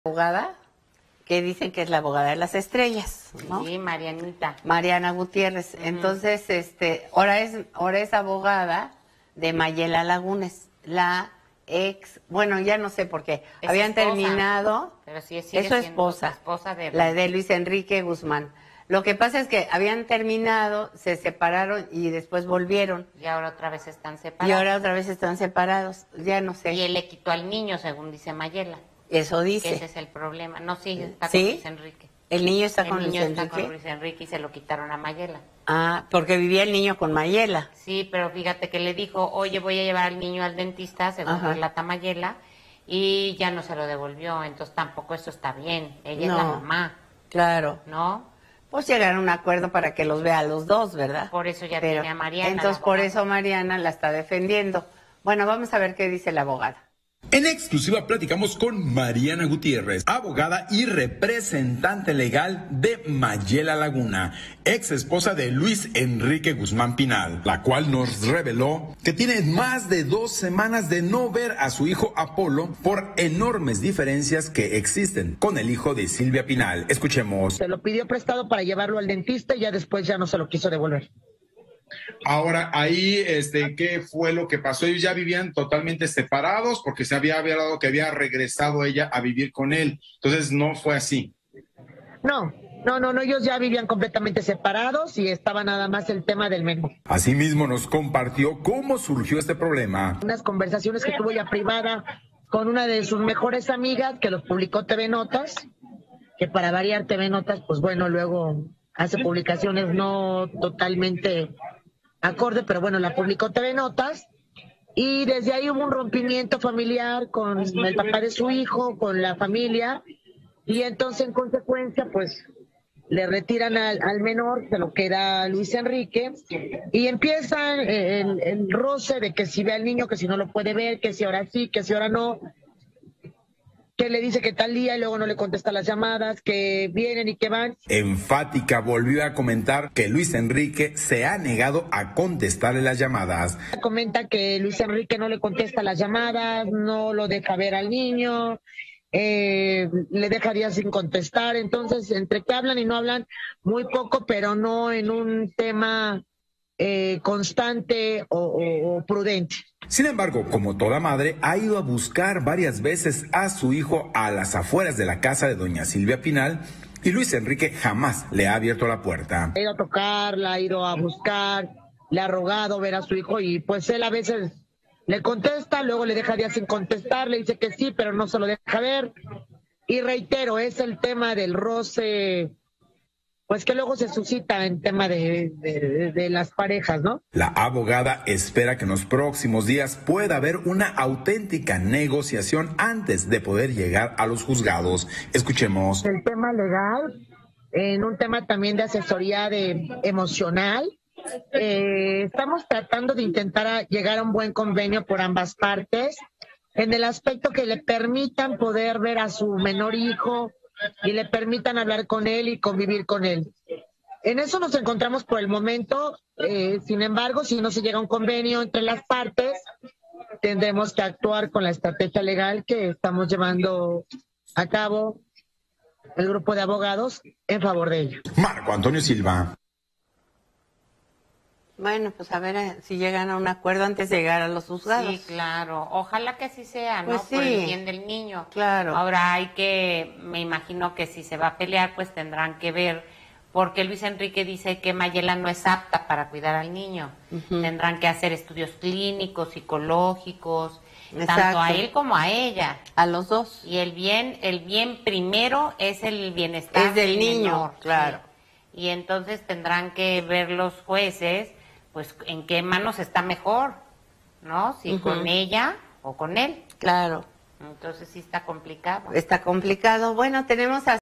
Luis Enrique Guzmán separó "con engaño" a su bebé de Mayela Laguna; abogados ya intervienen
Luis Enrique Guzmán, de la familia Pinal, encabeza una pelea con su esposa Mayela Laguna que involucra la sustracción de su hijo en común. De acuerdo con la abogada, Guzmán se llevó al bebé con pretexto de ir al dentista y desde entonces no le permite a la madre ver a su hijo.
Esta pareja está en controversia desde que la revista TV Notas publicará unos audios privados de Laguna arremetiendo contra su esposo.

Canal sugerido

Descarga nuestra